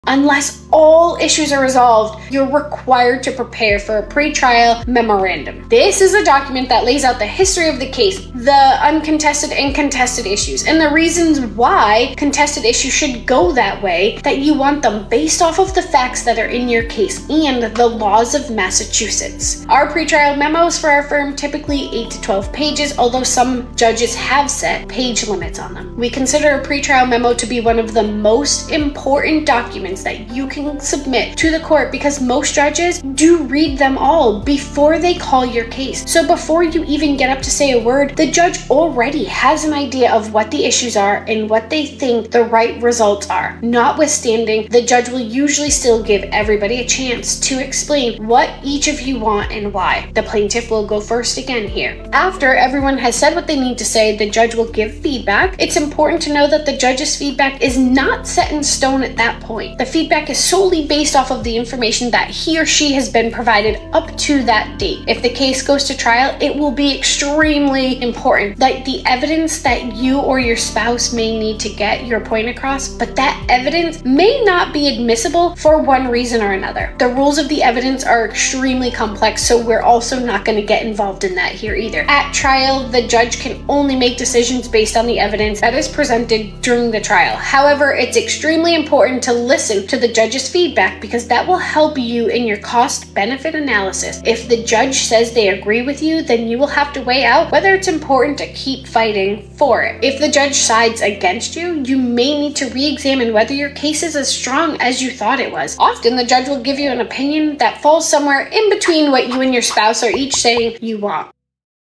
Your Pre-Trial Memorandum- What is it?
What is a pre-trial memorandum in the divorce process? Client Support Coach Stacey explains!